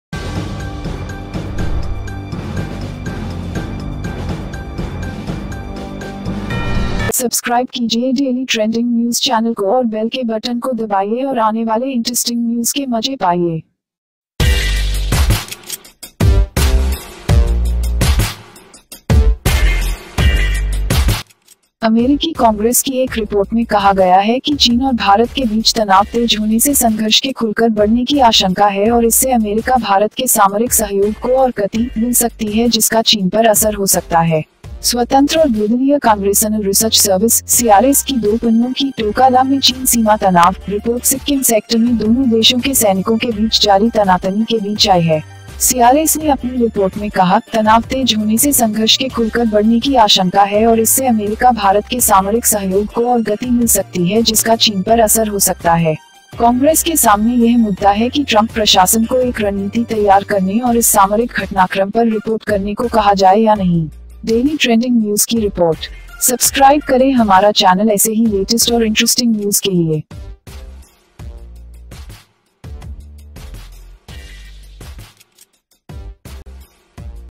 अमेरिकी रिपोर्ट में खुलासा, चीन-भारत युद्ध होकर रहेगा - अमेरिका करेगा भारत का समर्थन
अमेरिकी कांग्रेस की एक रिपोर्ट में कहा गया है कि चीन और भारत के बीच तनाव तेज होने से संघर्ष के खुलकर बढ़ने की आशंका है और इससे अमेरिका-भारत के सामरिक सहयोग को और 'गति' मिल सकती है जिसका चीन पर असर हो सकता है.